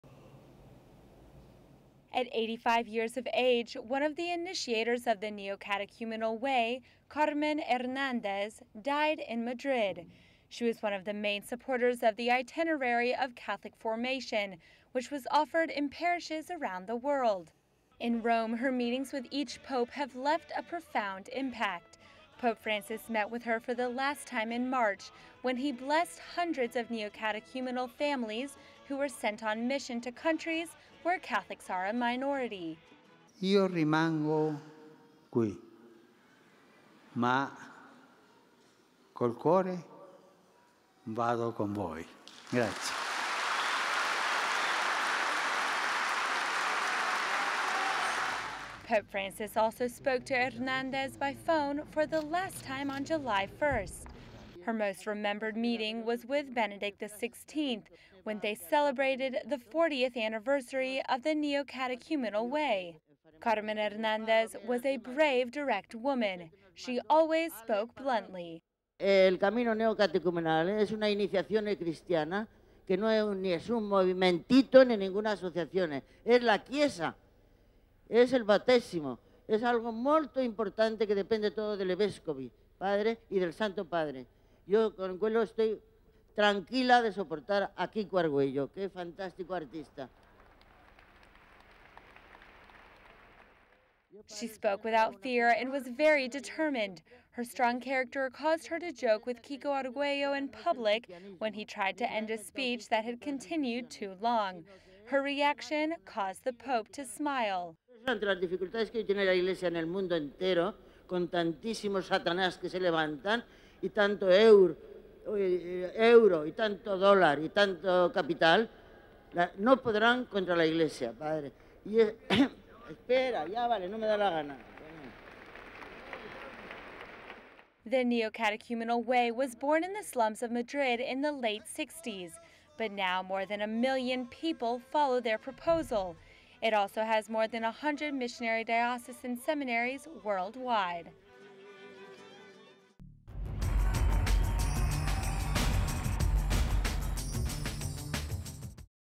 Initiator of the Neocatechumenal Way, Carmen Hernandez, passes away
She, Kiko Arguello and Mario Pezzi began one of the Church's most dynamic realities of the century.